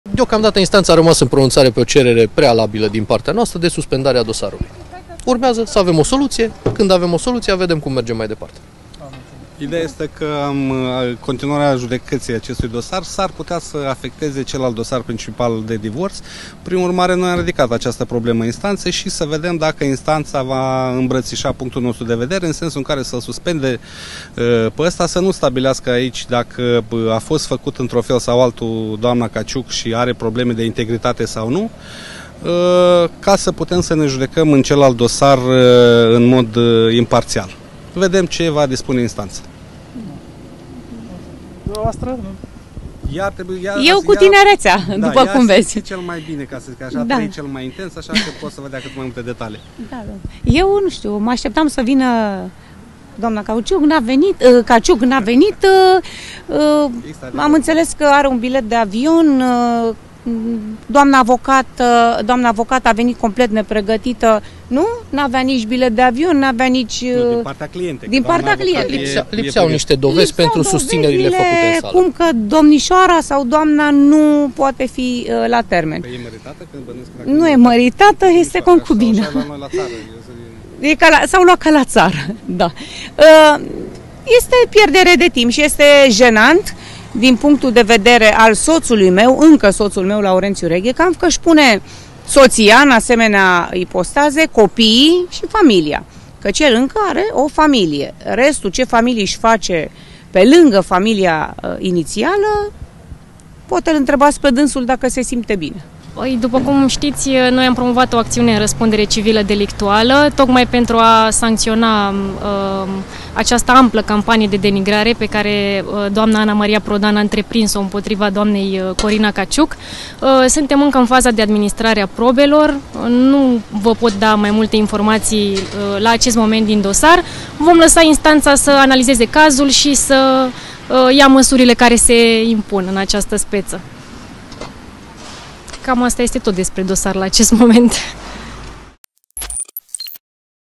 S-a reaprins SCANDALUL! 💣 Corina Caciuc a dat-o în judecată pe Anamaria Prodan! E război cât casa!
Corina Caciuc, iubita lui Laurențiu Reghecampf, a acuzat-o pe Anamaria Prodan de denigrare, iar vineri a avut loc un alt termen al procesului. În fața instanței, sexy-partenera antrenorului a fost reprezentată de avocat, iar CANCAN.RO vă prezintă, în exclusivitate, primele declarații ale părților implicate.

Anamaria Prodan nu a ratat nici acum ocazie să-și atace (încă) soțul și pe iubita acestuia, semn că nu s-a împăcat nici până acum cu ideea despărțirii.